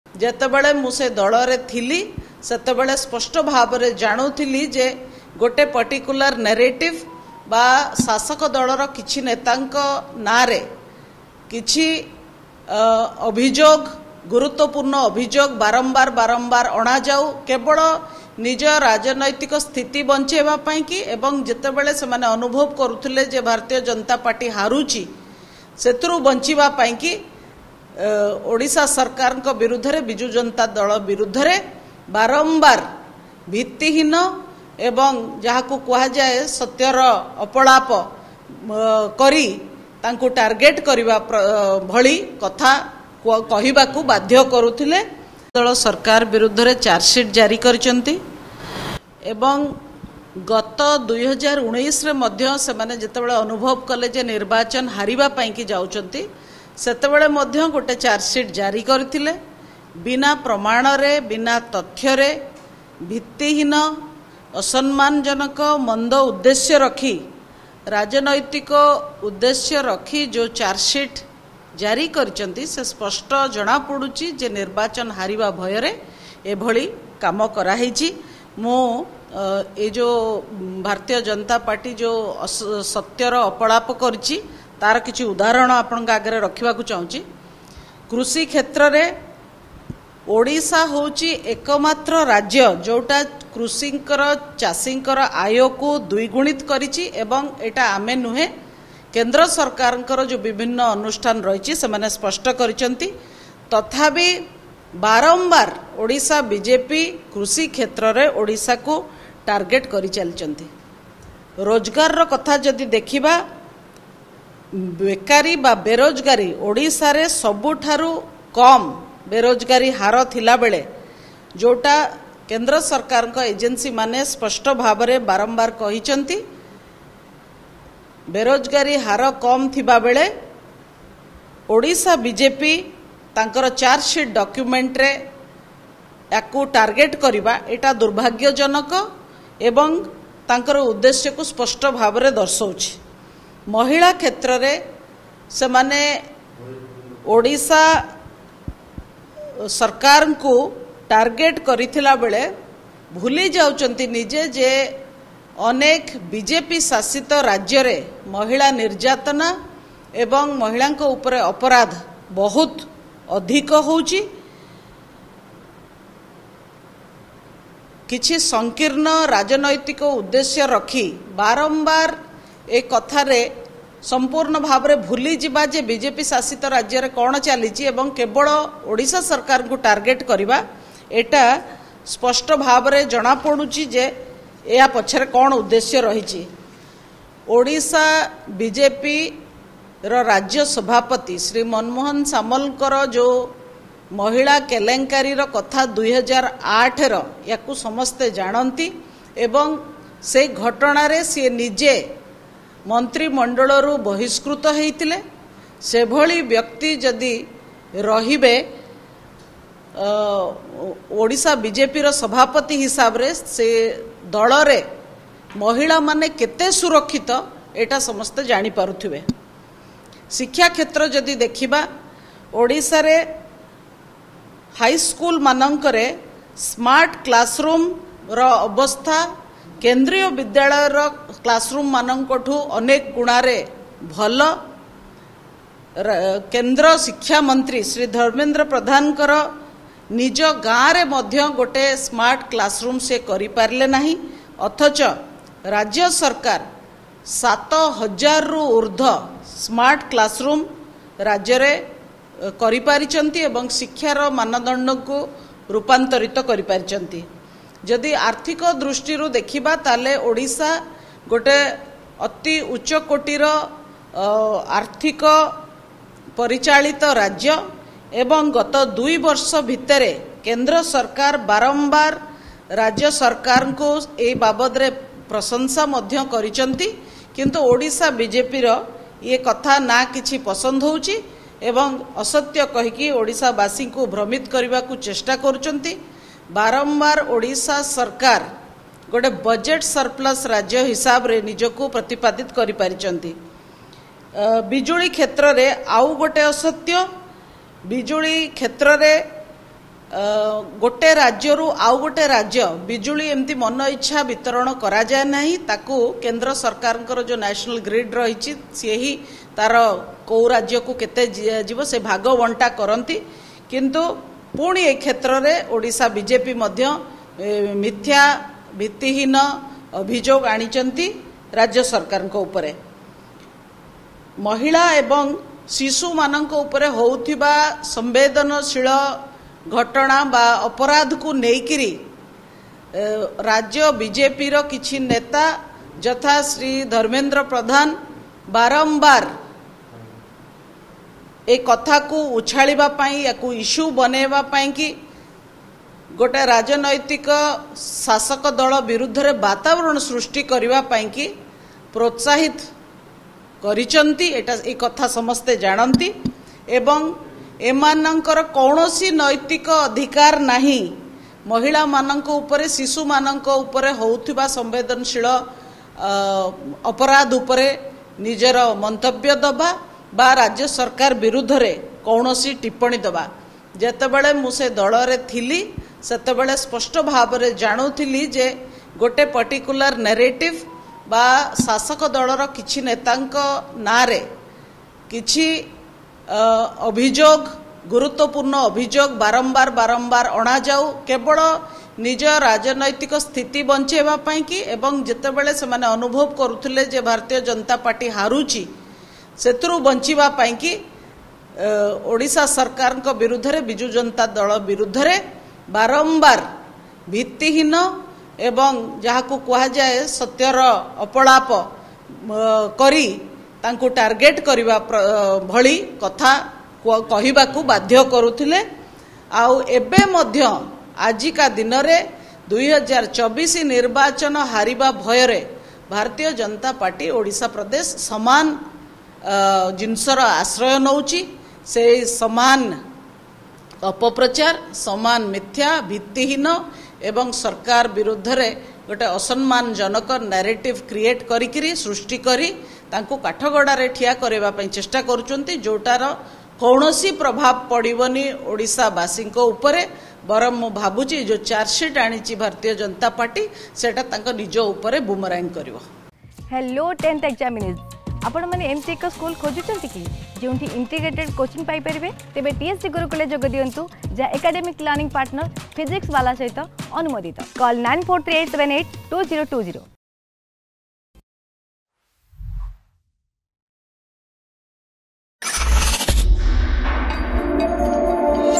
ବିଜେପି ମଧ୍ୟରେ ଥାଇ ଯାହା ଜାଣିଥିଲେ ସେହି କଥାକୁ ନେଇ ସବୁ ଗୁମର ଖୋଲିଲେ ଲେଖାଶ୍ରୀ || Political News
Sunstar TVoneof theBest WebMedia Youtube Channel,our team try to collect latest news,social message